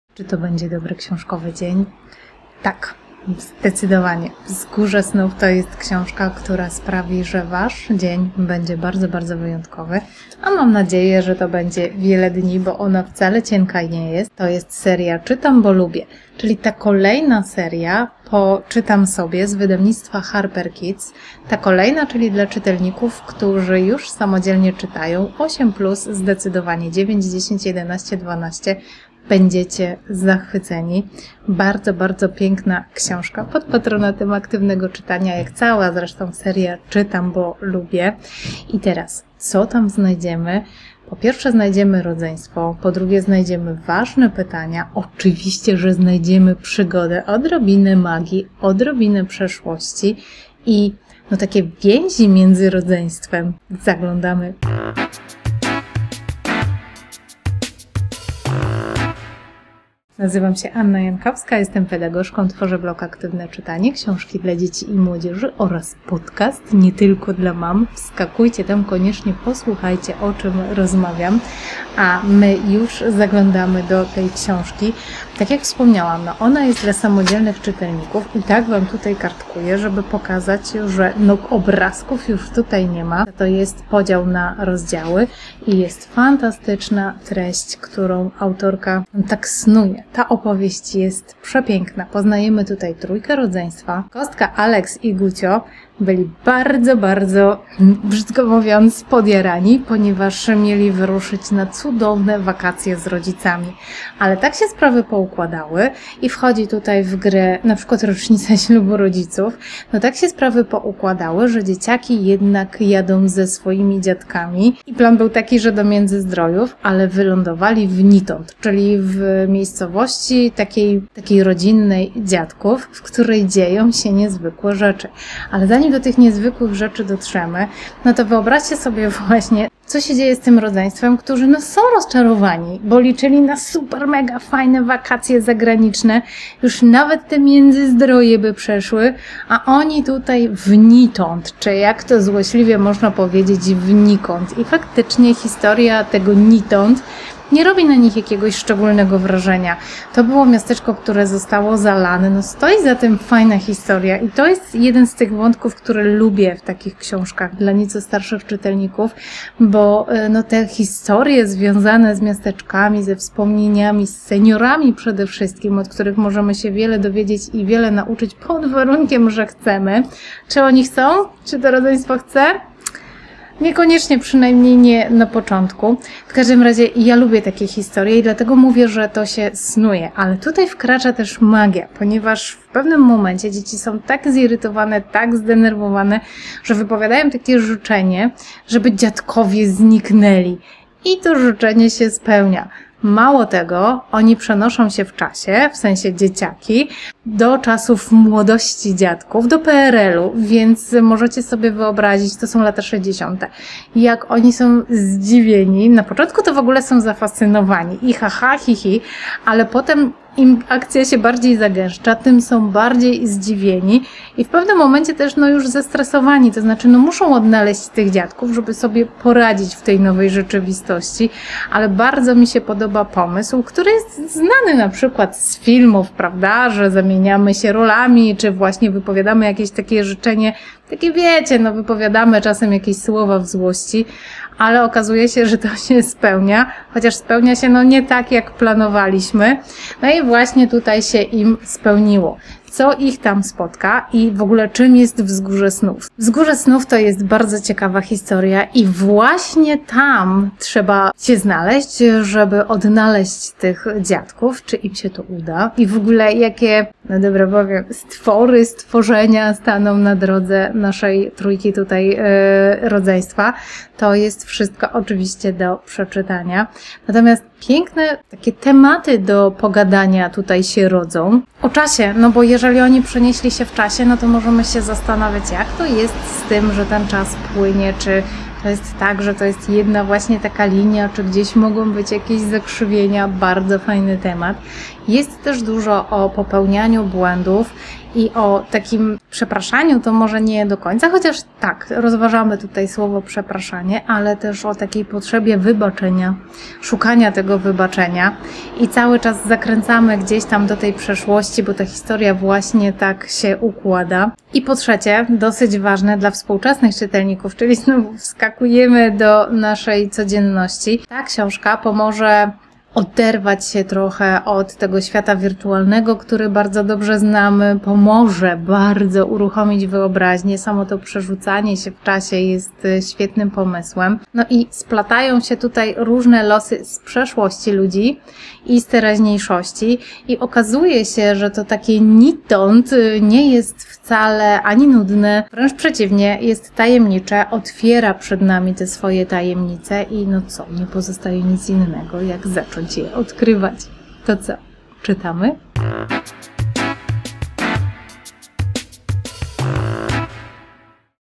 Wzgórze Snów. Nowa książka z serii „Czytam, bo lubię”.
Zdarzyło ci się wypowiedzieć na głoś życzenie (lub po prostu słowa w złości), które się spełniło? Oj…

Dzisiaj zabiorę cię do miejsca, którego już nie ma. Dokąd dokładnie? Do Nitąd! A właściwie zabierze nas tam Joanna Jagiełło na kartach swojej książki „Wzgórze Snów” od wydawnictwa HarperKids.

Nowa książka z serii „Czytam, bo lubię” po patronatem Aktywnego Czytania.

Jesteśmy gotowi? Mam nadzieję, że tak, bo autorka tej książki jest wspaniała Joanna Jagiełło. Ona potrafi w przygody i emocje!

Kostka, Gucio i Aleks to rodzeństwo. Aleks jest najstarszy, 6-letni Gucio najmłodszy, ale to z perspektywy Kostki wkraczamy do tej historii.

Mieszkają z rodzicami i niezbyt rozgarniętą yorczką Mufką w Warszawie i wiodą tam całkiem dostatni żywot.

Właśnie nadeszły wakacje i cała rodzina szykuje się do wyjazdu na Lanzarote. Tyle że plany nagle ulegają zmianie.
Rodzice uznają, że skoro zbliża się ich rocznica ślubu, chcieliby jednak wyjechać sami. W zamian organizują dzieciom wypad do Międzyzdrojów z dziadkami.

Zatłoczony, polski kurort zamiast podróży na Kanary? Zdaniem rodzeństwa to nie może się udać! Jak niby Kostka miałaby się czymś takim pochwalić przed followersami na swoim kanale na YouTubie?!

Ale to jeszcze nie wszystko. Okazuje się, że może być gorzej. Babcia i dziadek też mają w nosie Międzyzdroje i postanawiają zabrać wnuki na wyprawę w swoje rodzinne strony. Do Nitąd, w którym oboje się wychowali. Do Nikąd, jak złośliwie zauważa Aleks.
A żeby było jeszcze trudniej, wybierają się tam kamperem.

Kostka i jej bracia nie szczędzą dziadkom przykrych słów. Atmosfera się zagęszcza, pojawiają się konflikty. W końcu rozgoryczone dzieciaki wypowiadają wieczorem życzenie: żeby dziadkowie zniknęli. Rano okazuje się, że tak właśnie się stało.

Chcesz wiedzieć, co wydarzyło się dalej? Przeczytaj cała recenzję na blogu Aktywne Czytanie.